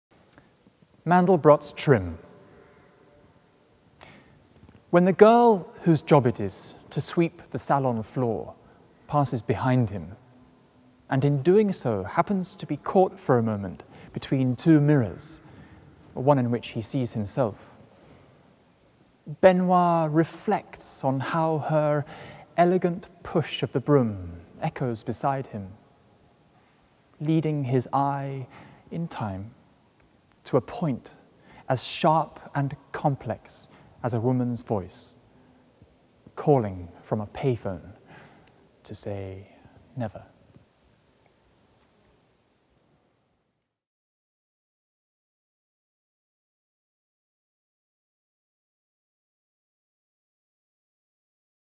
'Mandelbrot's Trim' - Theo Best
Theo Best, poet and actor, performs 'Mandlebrot's Trim'.

Theo took part in the live literature residency Set, where he developed his work along with four other promising poets.

Set was developed by Writers' Centre Norwich and Apples and Snakes.

This film was created by Brief Media.